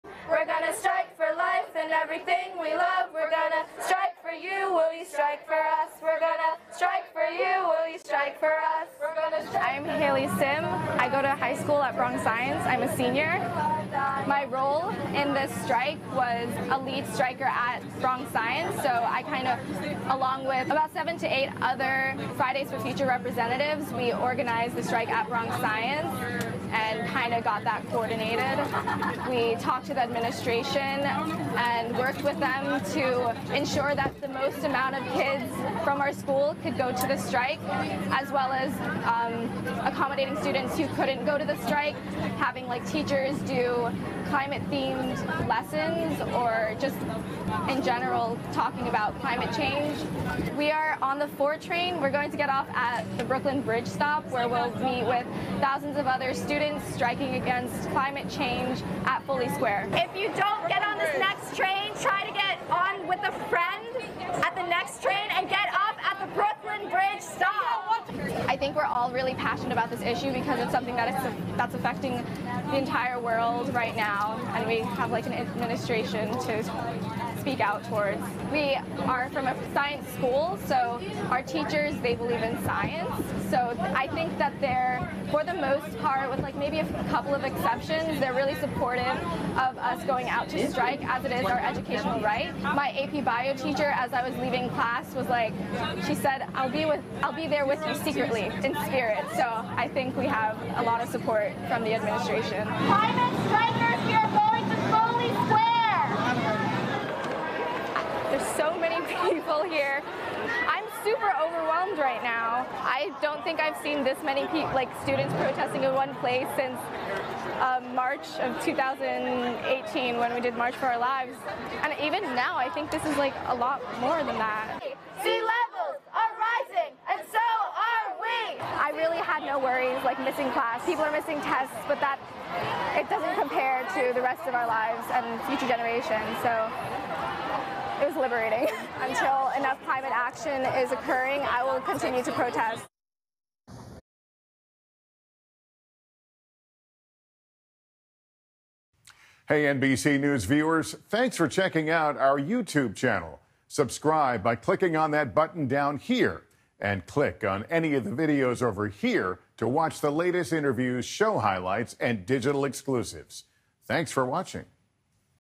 Meet The Students Walking Out Of School To March For Climate Change Action | NBC News Now
Students are walking out of classrooms in droves to participate in the climate change marches across New York City. NBC News follows one group from the Bronx High School of Science to get their reactions on what this protest means to them.